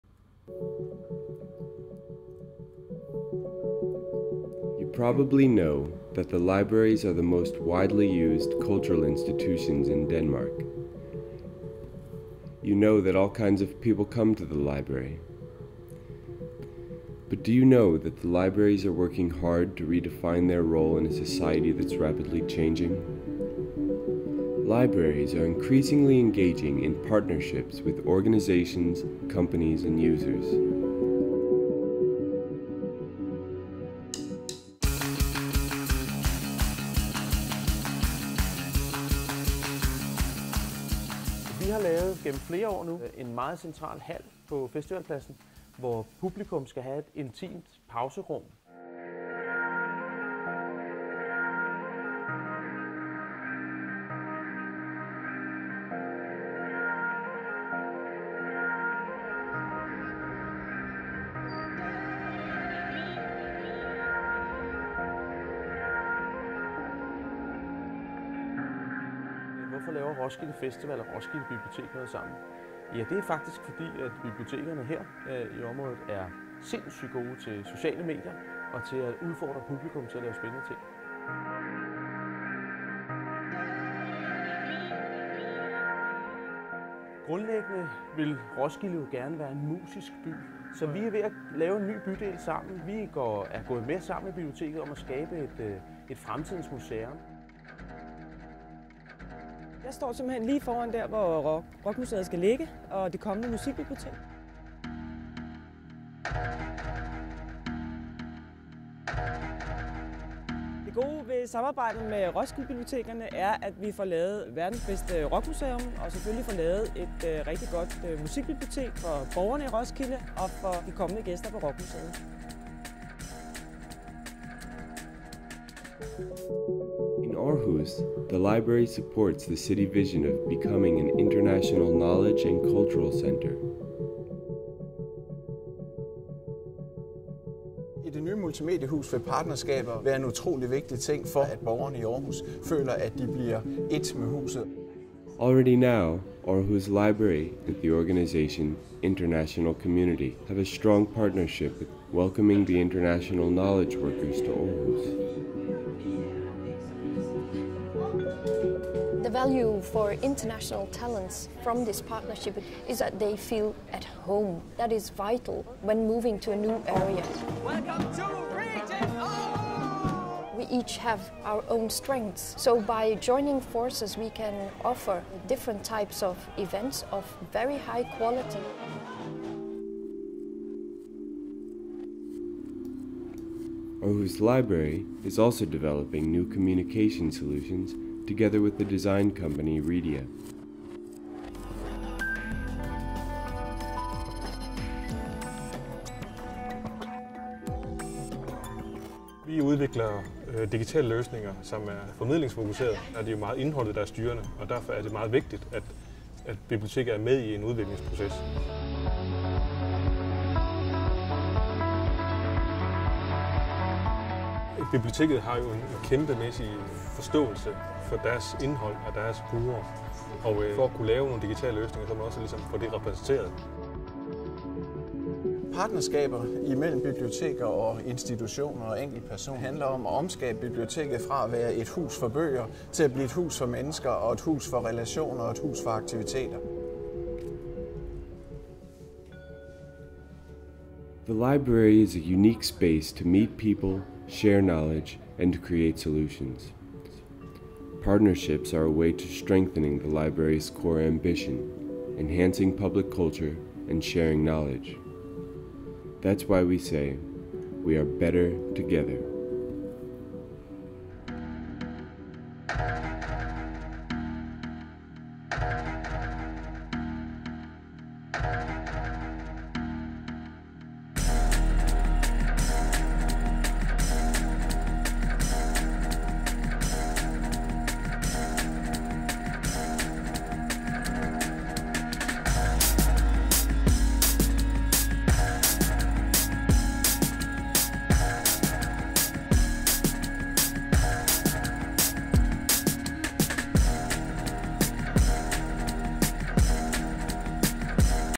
Better Together - The Potentials of Partnerships
Better Together is a short film about the potentials of partnerships between libraries and organisations, companies and users. The film introduces examples from Roskilde and Aarhus.